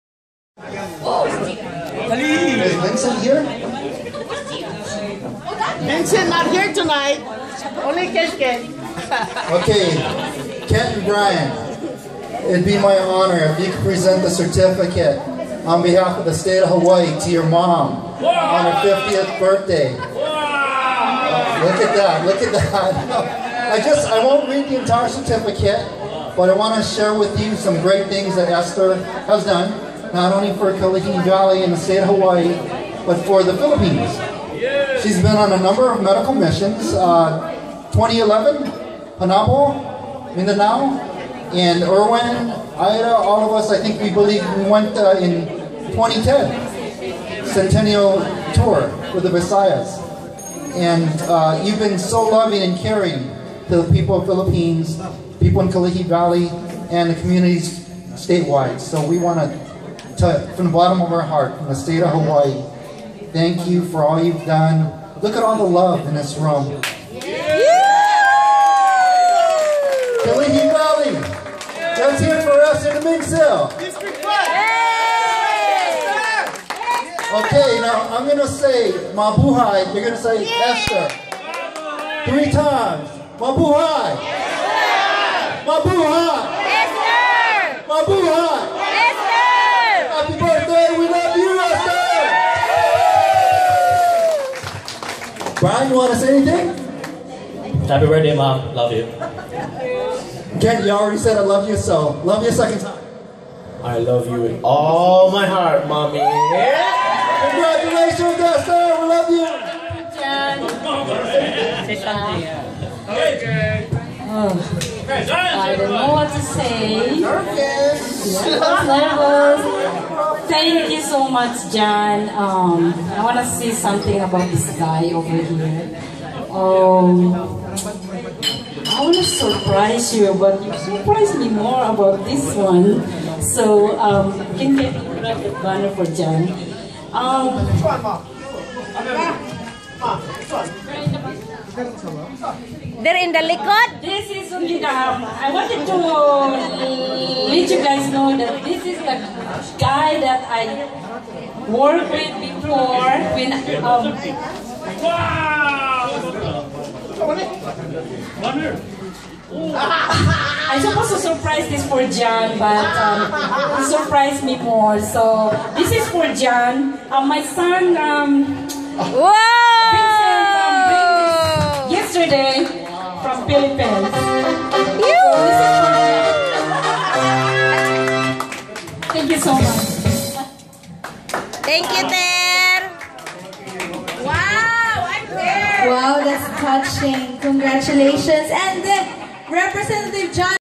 Mizuno presents a Certificate to honor Esther Domingcil at her 5-0 Birthday
Mizuno presents a Certificate from the state of Hawaii to honor Esther Domingcil at her special 5-0 Birthday and Esther presents a banner to Mizuno with photos of them in Mizuno's first election back in 2007.  According to Mizuno, "I was going to honor Esther and she ends up honoring me. Wow, we love Esther and our Ohana in Kalihi - This is how we are, one family one community "We are One."